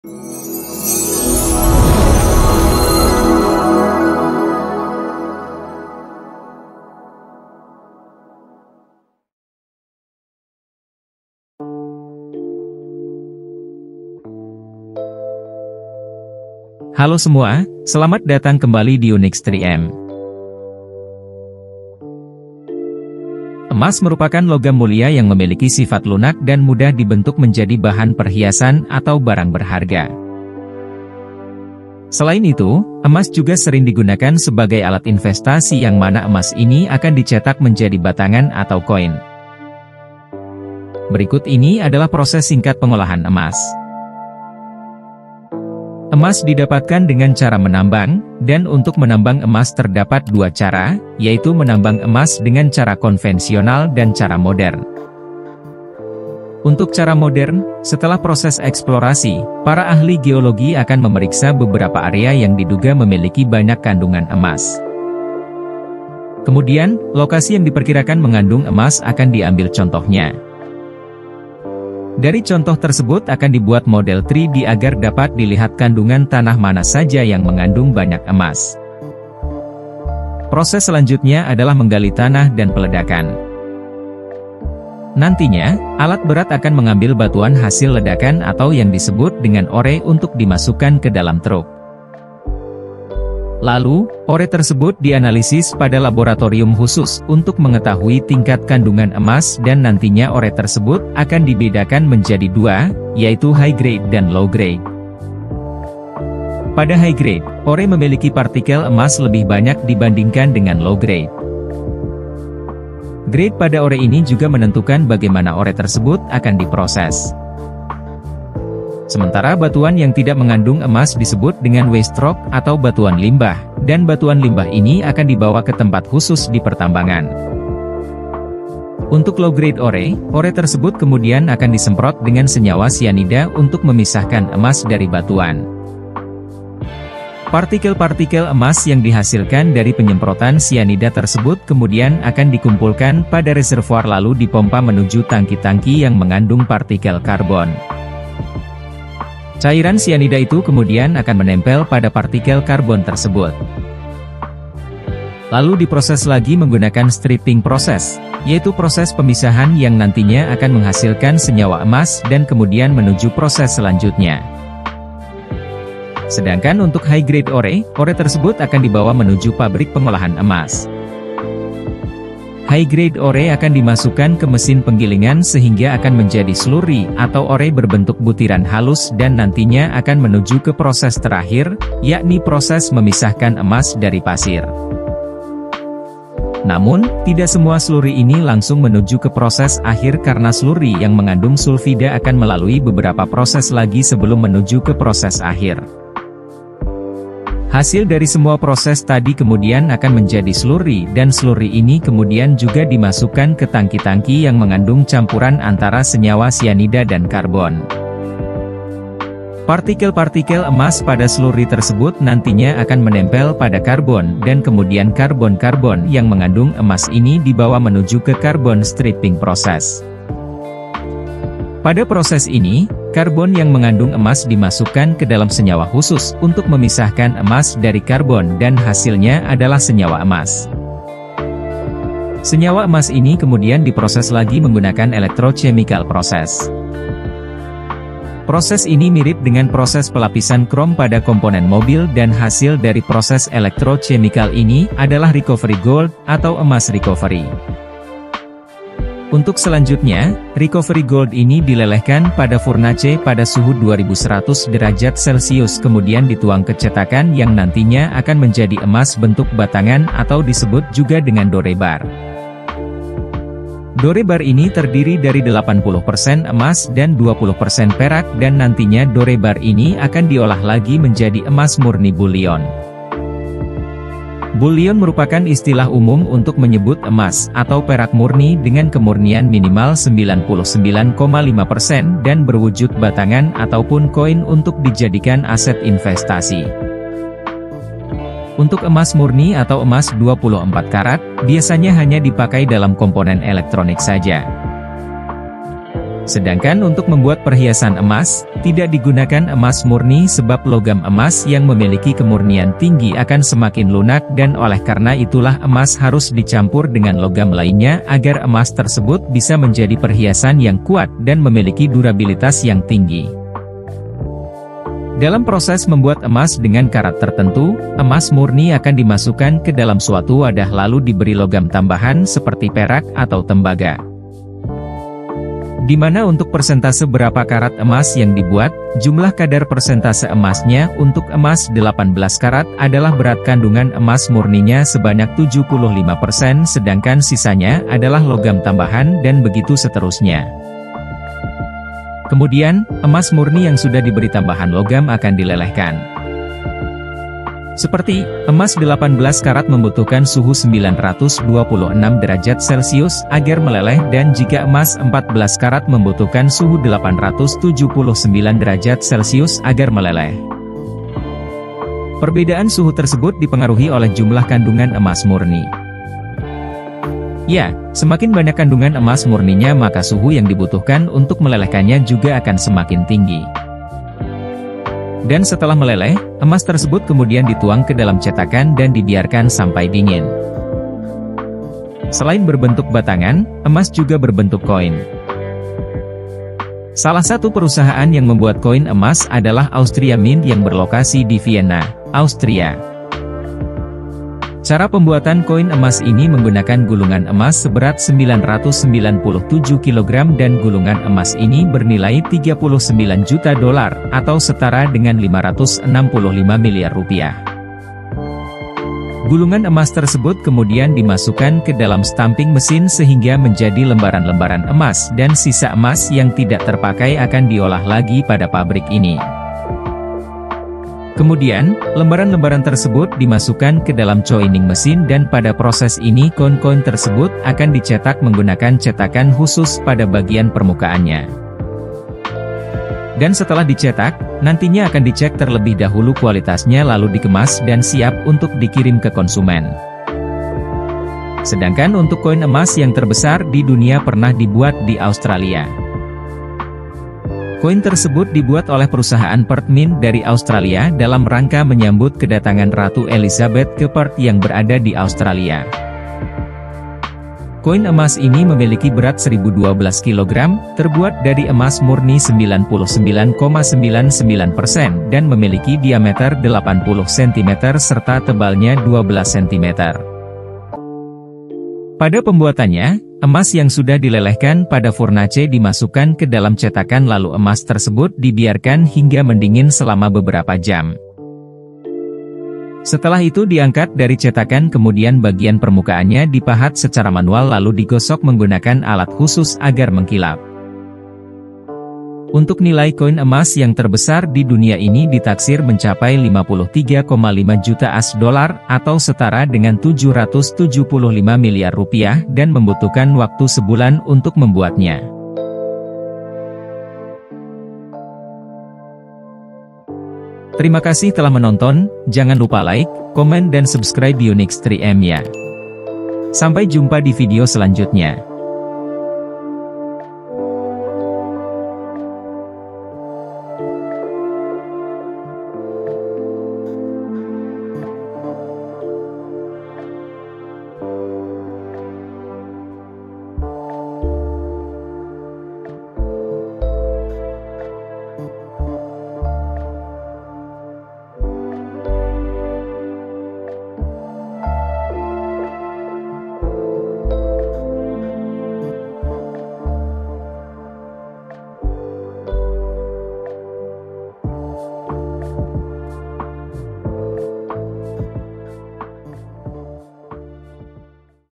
Proses Pembuatan Emas: Dari Tambang Hingga Batangan dan Koin
Selamat datang di saluran kami! Pada video kali ini, kami akan mengajak Anda untuk melihat proses lengkap pembuatan emas, mulai dari tahap penambangan hingga menjadi batangan dan koin berharga. Emas, logam mulia yang memiliki daya tarik luar biasa, tidak hanya digunakan sebagai perhiasan tetapi juga sebagai alat investasi yang diminati oleh banyak orang.

Video ini akan memperlihatkan kepada Anda dua metode penambangan emas: metode konvensional dan metode modern. Ahli geologi akan melakukan eksplorasi dan pemodelan 3D untuk mengidentifikasi lokasi dengan kandungan emas yang tinggi. Selanjutnya, kita akan melihat proses penggalian tanah dan peledakan untuk mengambil bijih emas dari dalam bumi.

Bijih emas yang telah diambil akan diuji di laboratorium khusus untuk menentukan tingkat kandungan emasnya. Selanjutnya, bijih akan dibedakan menjadi dua kategori, yaitu hi-grade dan low-grade, tergantung pada jumlah partikel emas di dalamnya. Kami juga akan menjelaskan bagaimana batuan limbah atau waste rock dipisahkan dari bijih yang mengandung emas.

Proses pengolahan emas untuk low-grade ore melibatkan penggunaan senyawa sianida untuk memisahkan emas dari batuan. Kami akan menjelaskan secara rinci bagaimana partikel emas terpisah dan dikumpulkan menggunakan karbon. Sedangkan untuk hi-grade ore, bijih akan dibawa ke pabrik pengolahan emas dan melalui tahap penggilingan untuk menghasilkan slurry. Kami juga akan menjelaskan bagaimana emas dipisahkan dari pasir dan bagaimana slurry yang mengandung sulfida diolah lebih lanjut.

Setelah proses pemisahan, emas yang telah diperoleh akan berbentuk slurry. Tahap selanjutnya melibatkan penggunaan karbon untuk menempelkan partikel emas dan menghasilkan senyawa emas. Kami akan membahas proses stripping karbon yang melibatkan pemisahan emas dari karbon. Kemudian, senyawa emas akan diproses menggunakan metode electrochemical process untuk menghasilkan emas murni.

Video ini juga akan mengungkapkan tahap akhir pembuatan emas, di mana emas murni akan dilelehkan pada suhu yang sangat tinggi dan dituang ke dalam cetakan untuk membentuk batangan emas atau dore bar. Selanjutnya, dore bar akan diolah menjadi emas murni bullion yang memiliki kemurnian minimal 99,5 persen dan digunakan sebagai aset investasi.

Kami juga akan menunjukkan proses pembuatan koin emas. Anda akan melihat bagaimana Austria Mint, perusahaan yang terkenal dengan koin emasnya, menggunakan mesin khusus untuk mencetak koin emas dari lembaran emas. Kami juga akan membahas koin emas terbesar di dunia yang dibuat oleh Perth Mint dari Australia, dengan berat 1012 kg dan kemurnian 99,99%. Koin emas ini dipersembahkan untuk menyambut kedatangan Ratu Elizabeth ke Perth.

Nikmati perjalanan yang menarik ini dan lihat dengan mata kepala sendiri bagaimana emas, dari proses penambangan hingga menjadi batangan dan koin berharga, diciptakan.